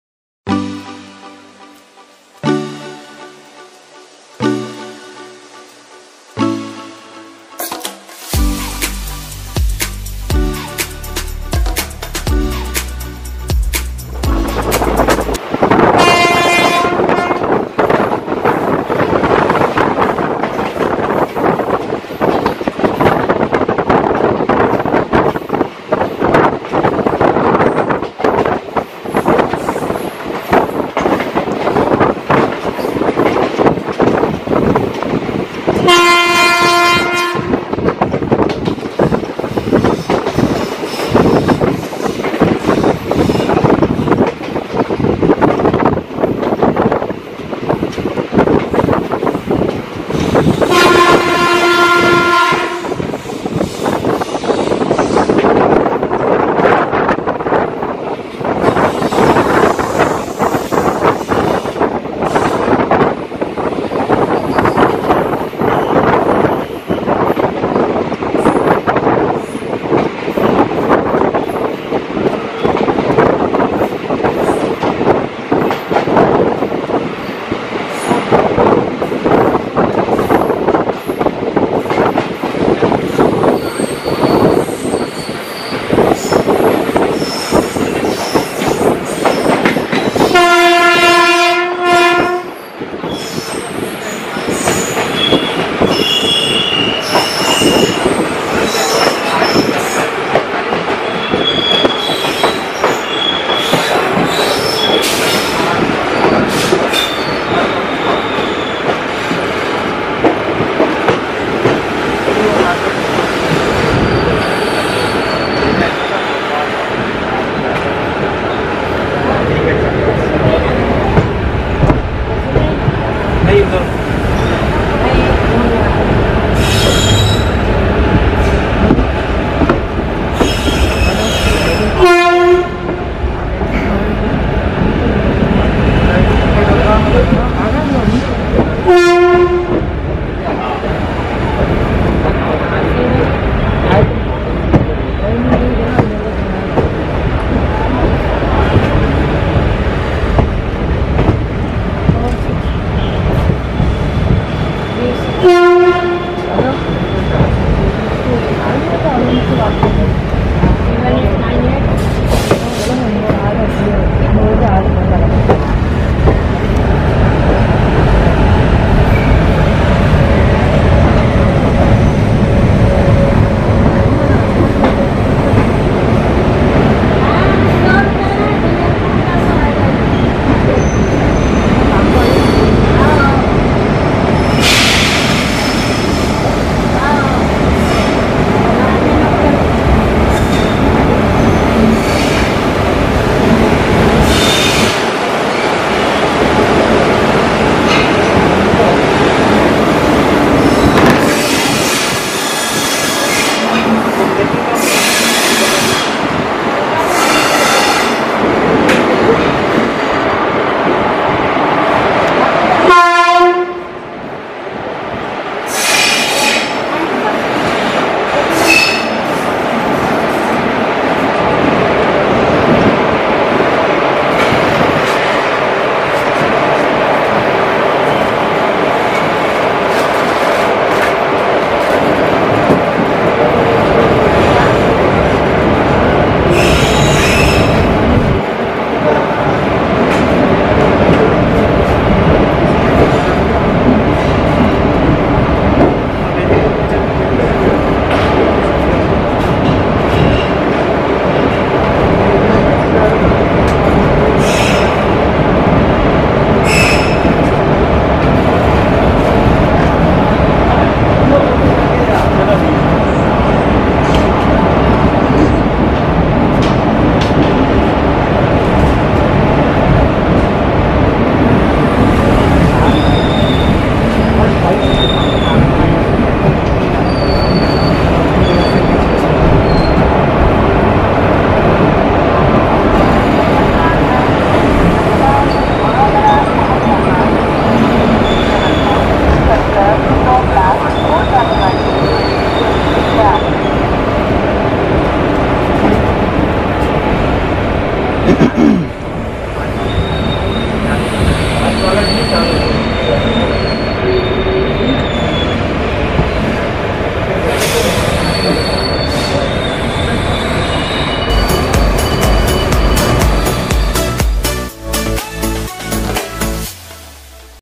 जबलपुर से इंदौर की सबसे लोकप्रिय ओवरनाइट स्पेशल भारत के सबसे स्वच्छ नगर इंदौर जंक्शन आगमन कर रही है
video taken in jan 2021